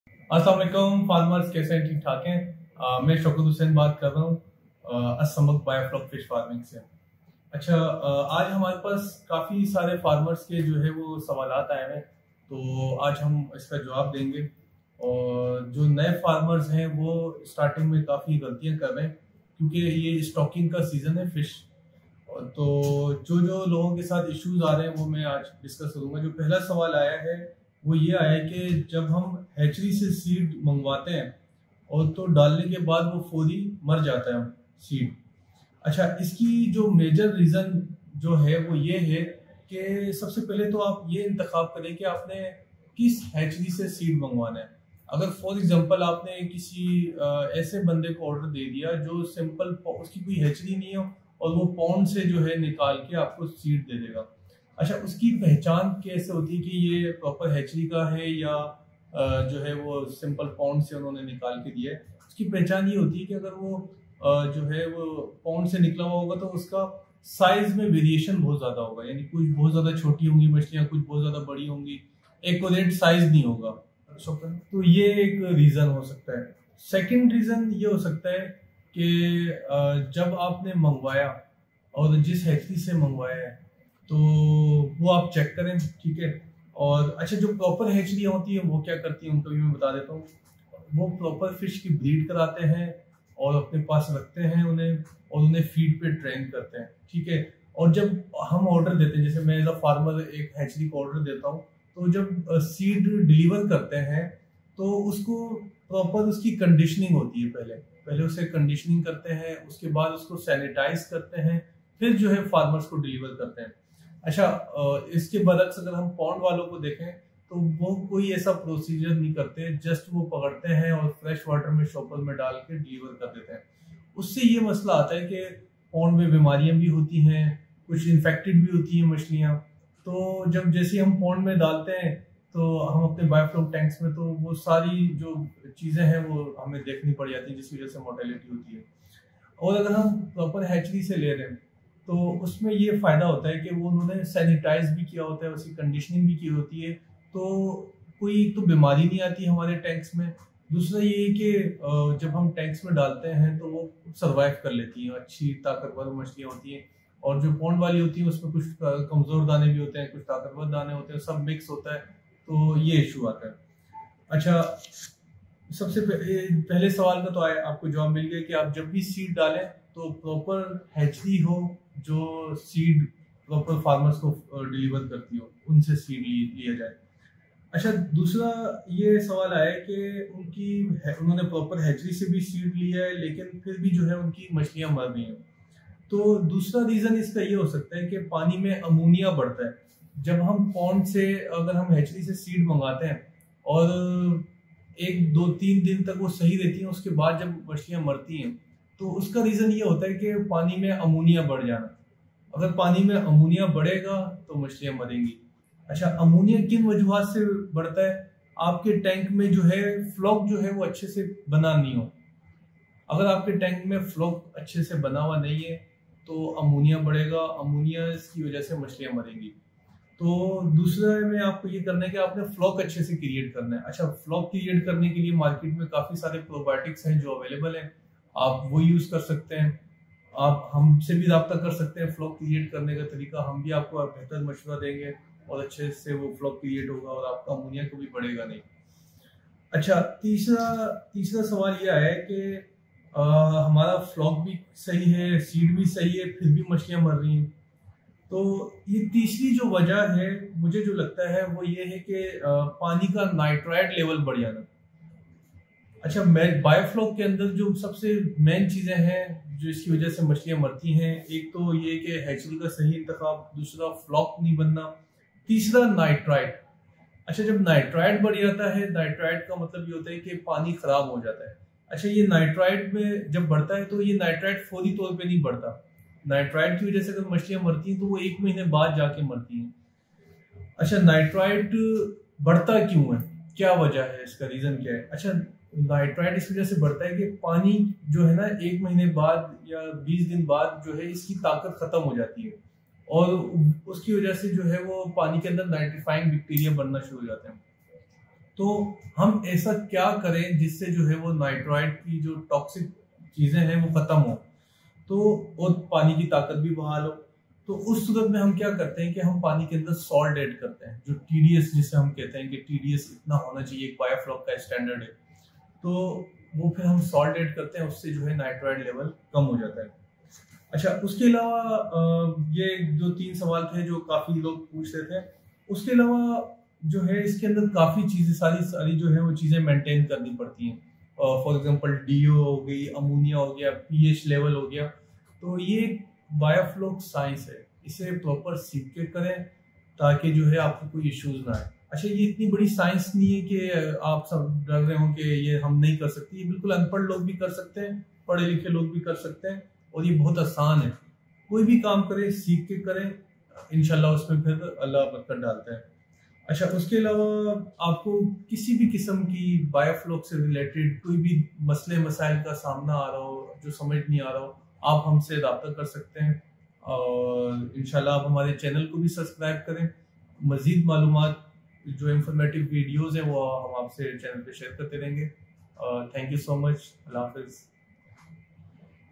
Why Fishes Dies in Biofloc Fish Farm Tank New Farmer's Problem Q/A By Assammak Farm
Today we cover Question that Frequently ask by the new Biofloc Fish farmers.